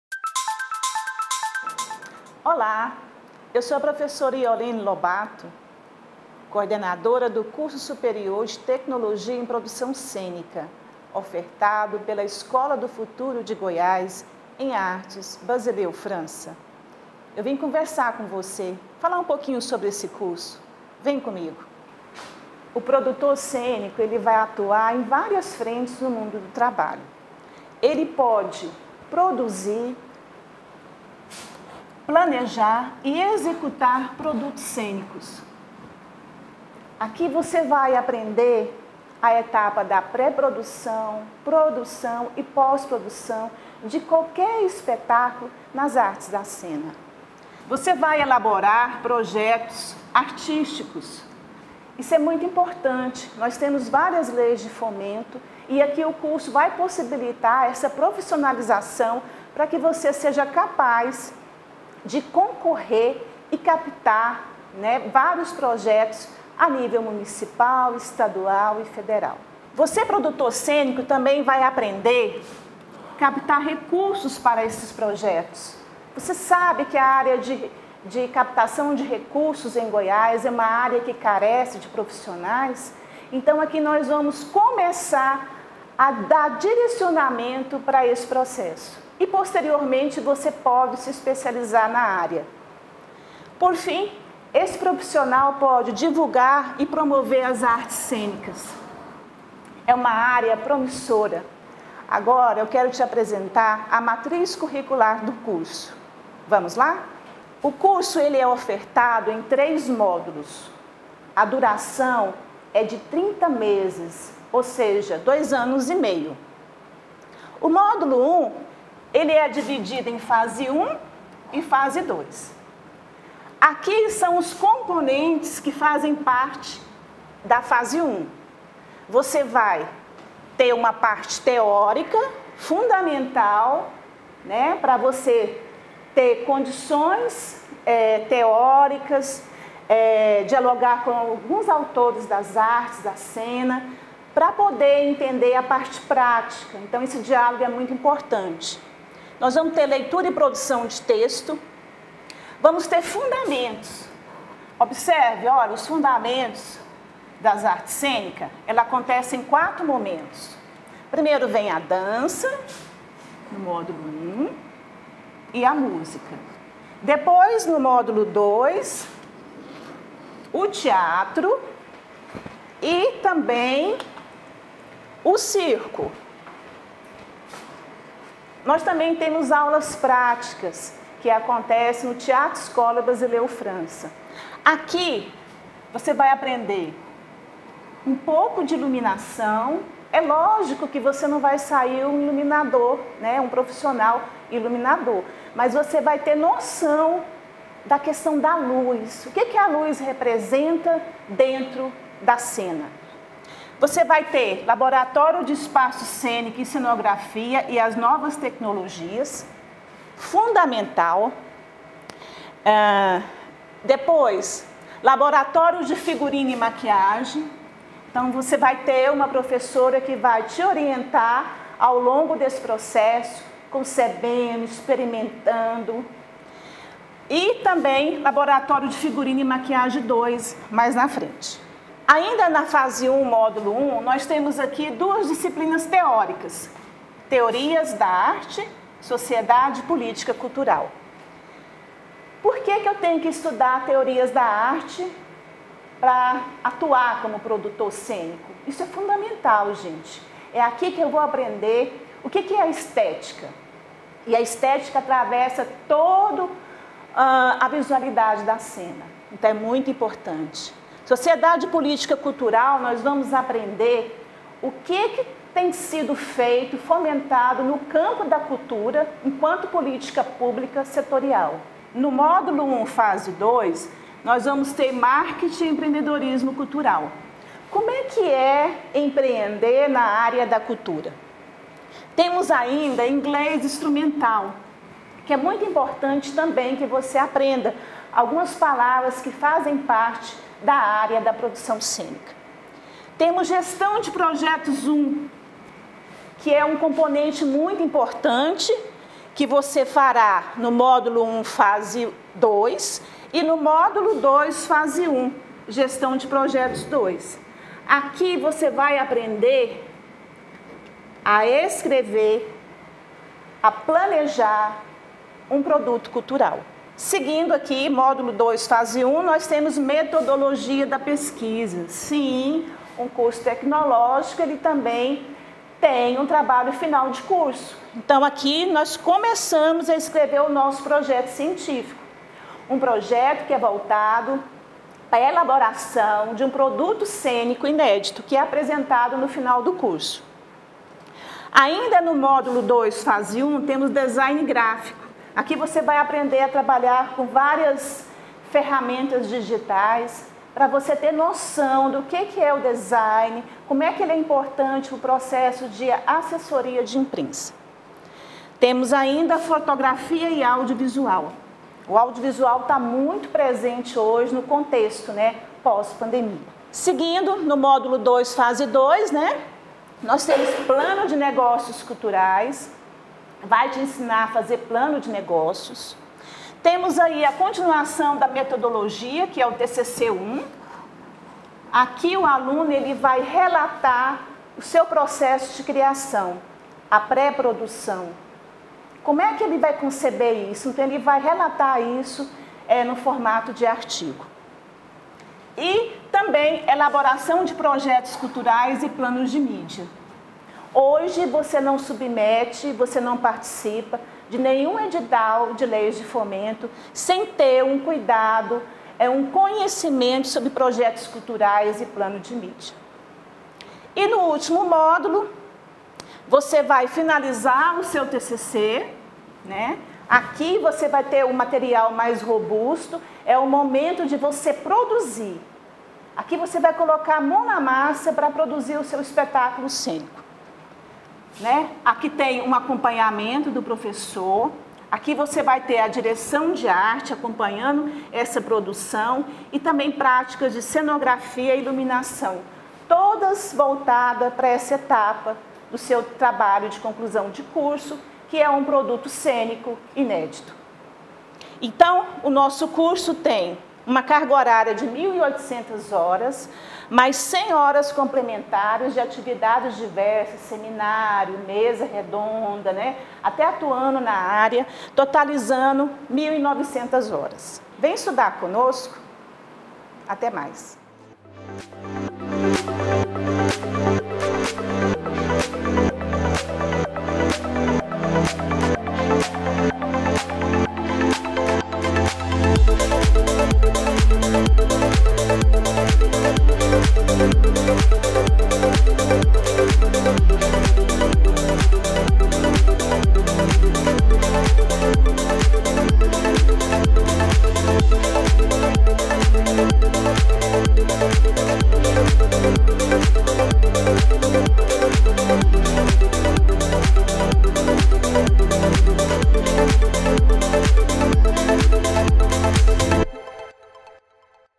APRESENTAÇÃO CURSO SUPERIOR EM PRODUÇÃO CÊNICA
A coordenadora do Curso Superior em Produção Cênica da Escola do Futuro em Artes Basileu França